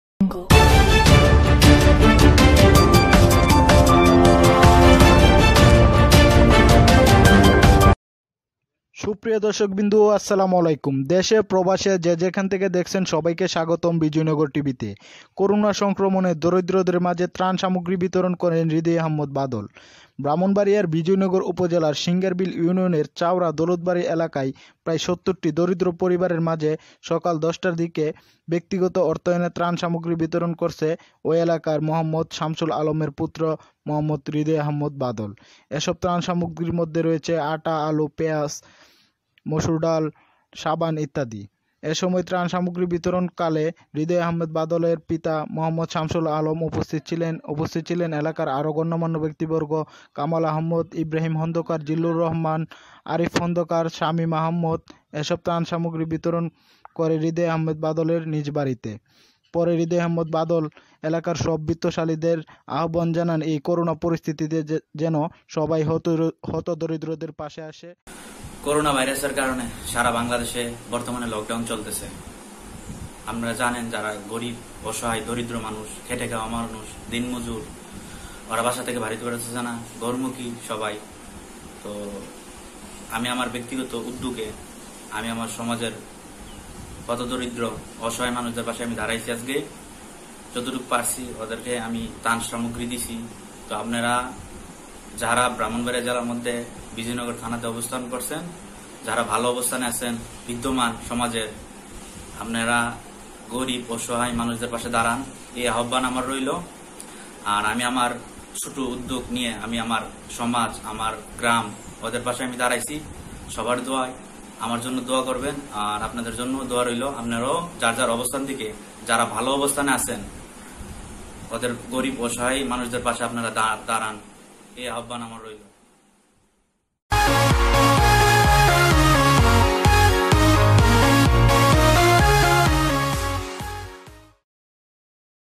সিংগারবিলের চাউরা দৌলত বাড়ী গ্রামে হৃদয় আহম্মেদ বাদলের ত্রান সামগ্রী বিতরণ।
সিংগারবিলের চাউরা দৌলত বাড়ী গ্রামে হৃদয় আহম্মেদ বাদলের নিজস্ব অর্থায়নে নিম্ন আয়ের ৭০ টি পরিবারের মধ্যে ত্রান সামগ্রী বিতরণ।